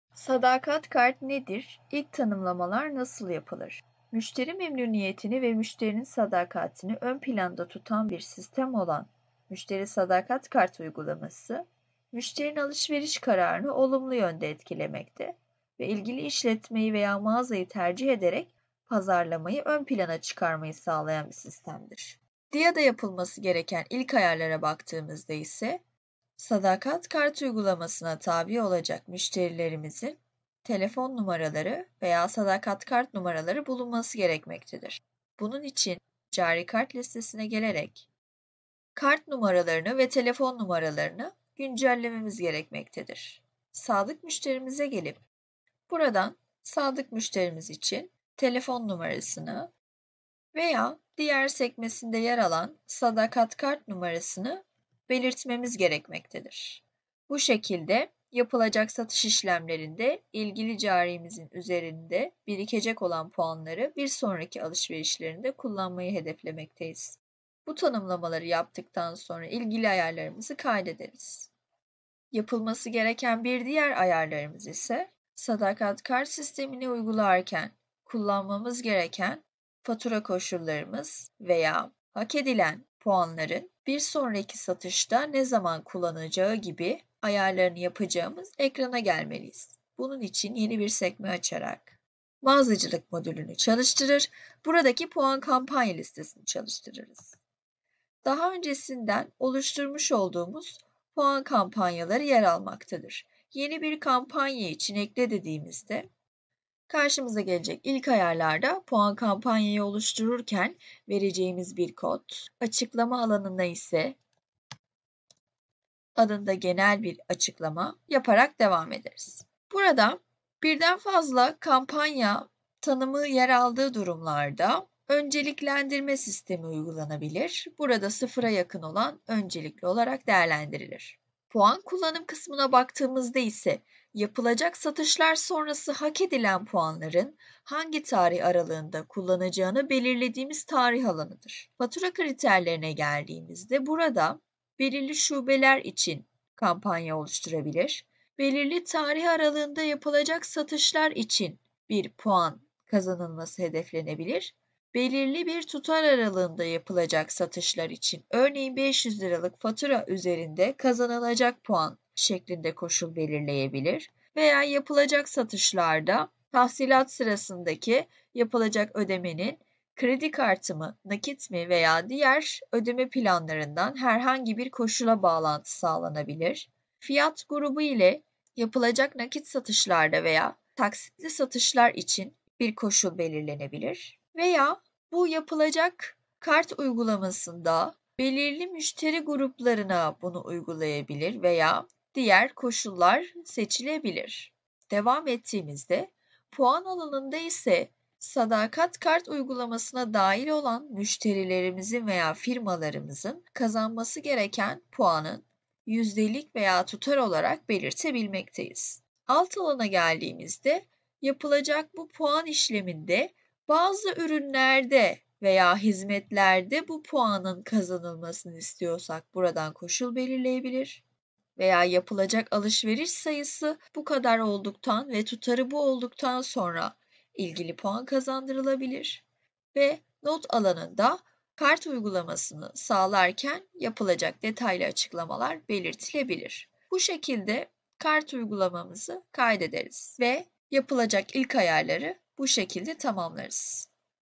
DİA Akademi - Müşteri Sadakat Kart Nedir? Nasıl Tanımlanılır?
Bu videoda müşteri sadakat kartının ne olduğunu ve nasıl tanımlandığı anlatılmaktadır.